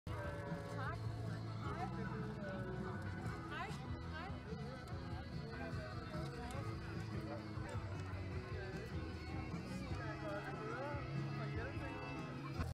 Hand kiss -Mette Fredriksen
Mette Fredriksen gets her hand kissed.